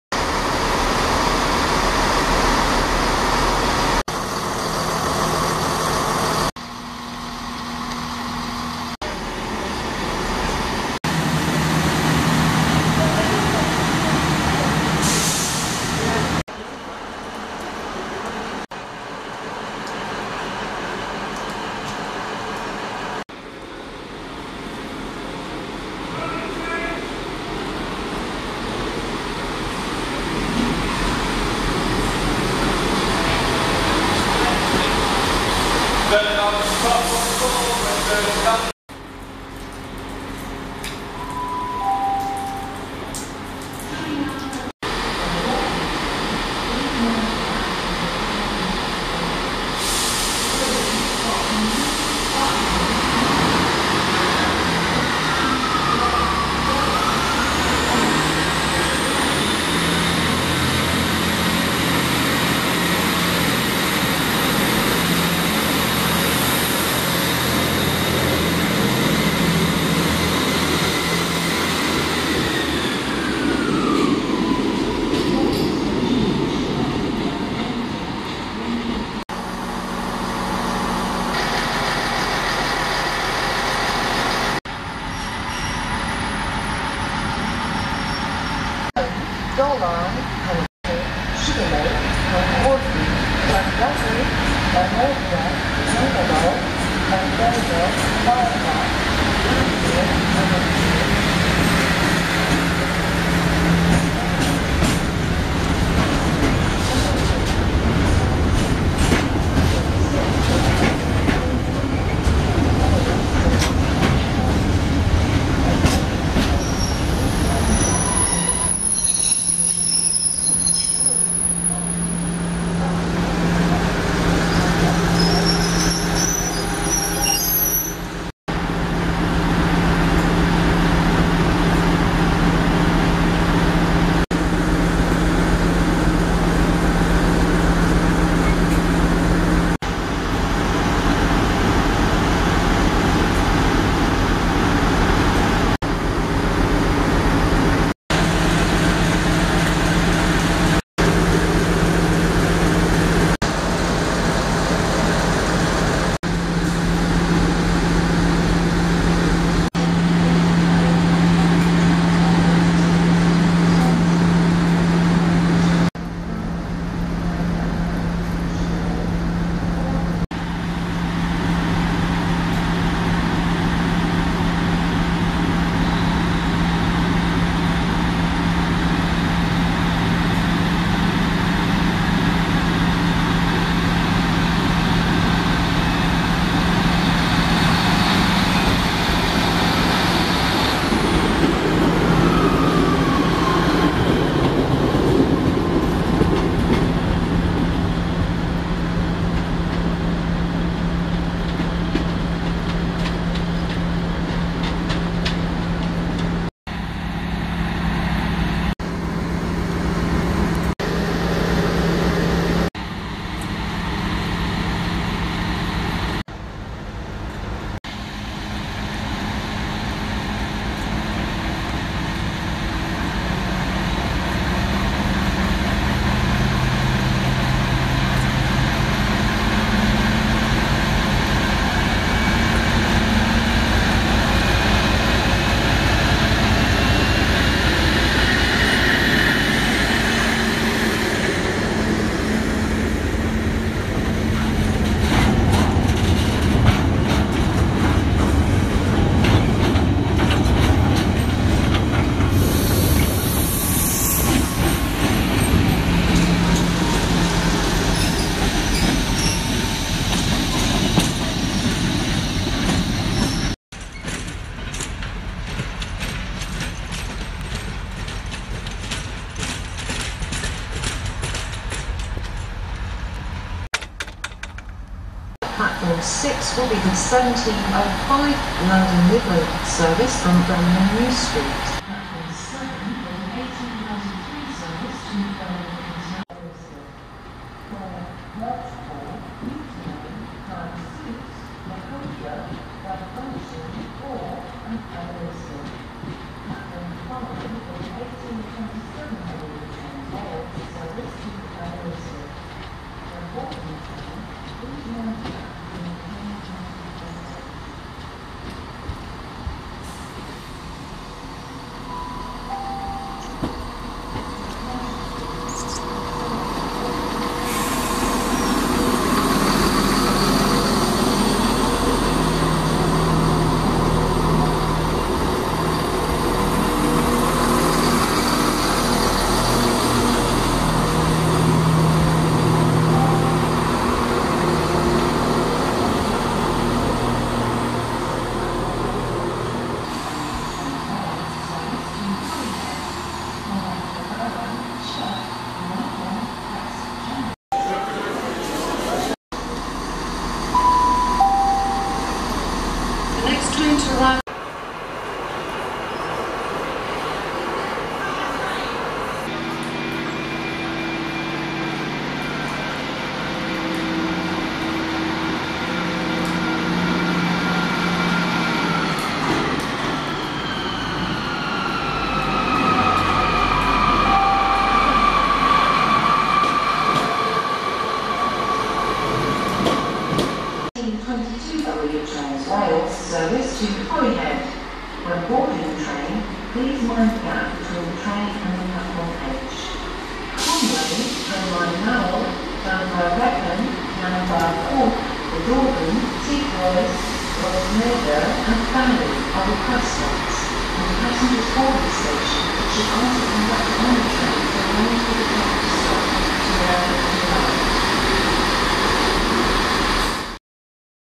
Trains at Shrewsbury December 2016
Trains at Shrewsbury Railway Station in December 2016 including Arriva Trains Wales, London Midland and an appearance from the RHTT  which headed South.